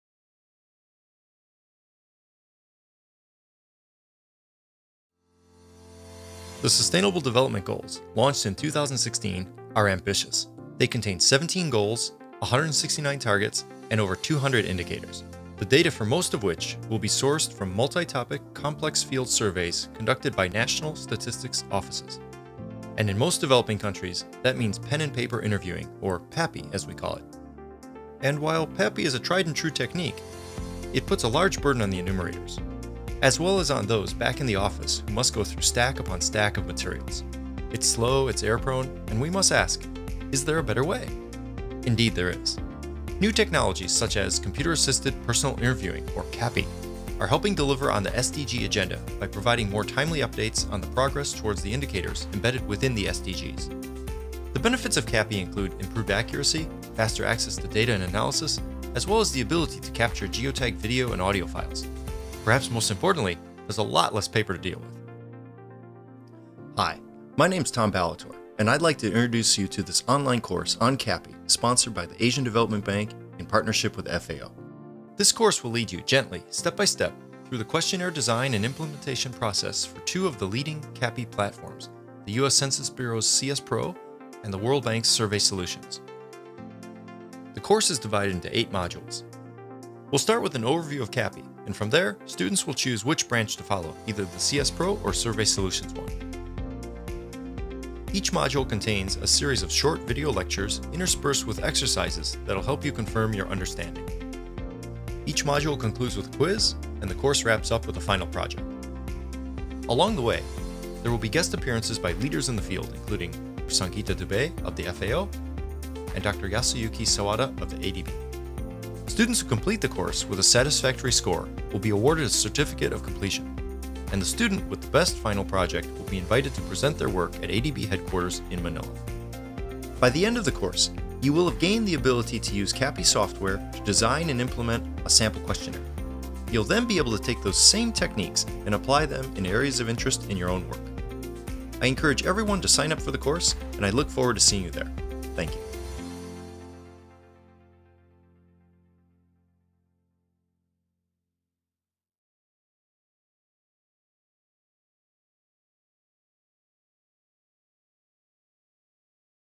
Promo Video for CAPI Course
Promotional Video for "Computer-Assisted Personal Interviewing (CAPI) for National Surveys in Support of the SDGs" online course sponsored by the Asian Development Bank (ADB) and the Food and Agriculture Organization of the United Nations (FAO) under the Global Strategy to improve agricultural and rural statistics (GSARS).